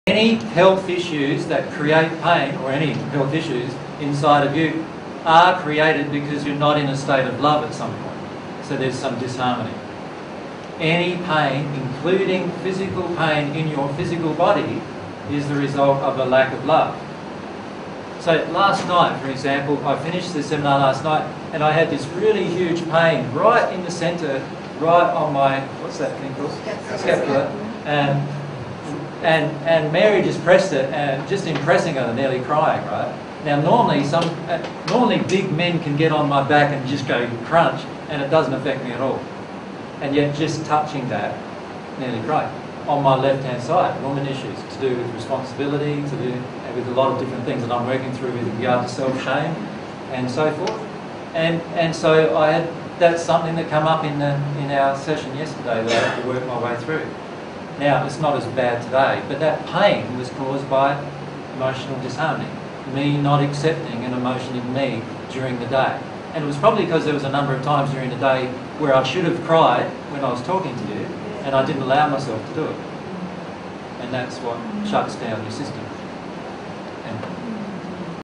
Jesus on Health Issues
Jesus touches on the relationship between physical health and feeling all of our emotions.

This is a section of the presentation by Jesus and Mary entitled "20090222 Human Relationships - Lessons In Natural Love S2P2".

dtstart:003917:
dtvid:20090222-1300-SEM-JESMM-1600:
dtmd5:6fc97774049bff64d5ad5e55f5ea8299: